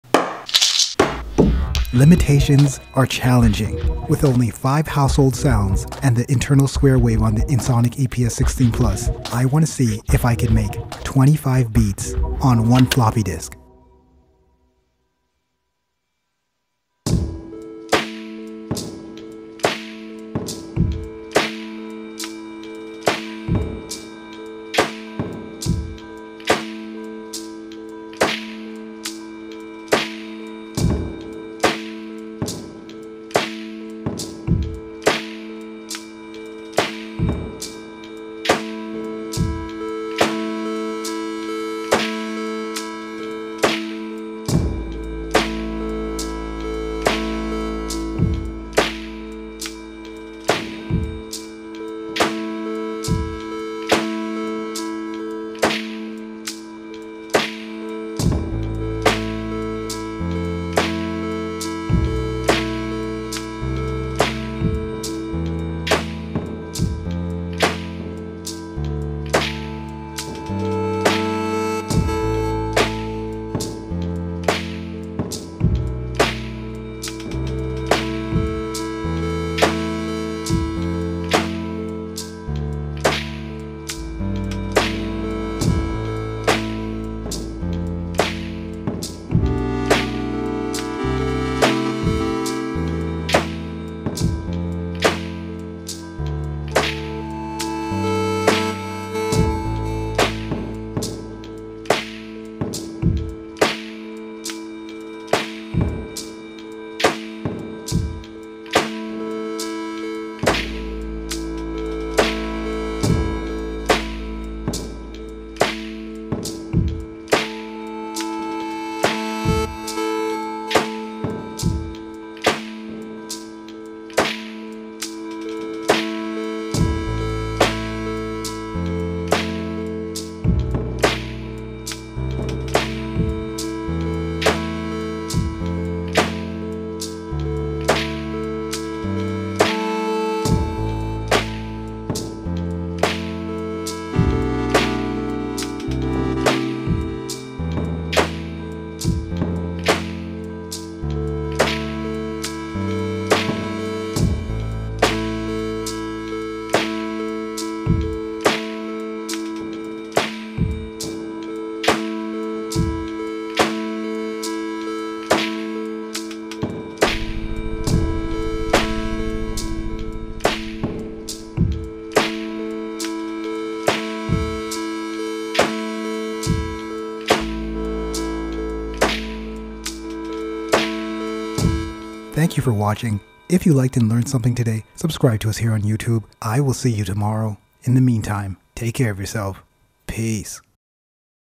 25 Beats On A Floppy – Day 13
The Ensoniq EPS16+ floppy disk holds 720kb of storage. With only five samples and one square wave, lets see if I can make 25 BEATS ON A FLOPPY. Today is day 13.

* Fun Fact: 1GB = 1388 floppy disks.

Peace,

TDS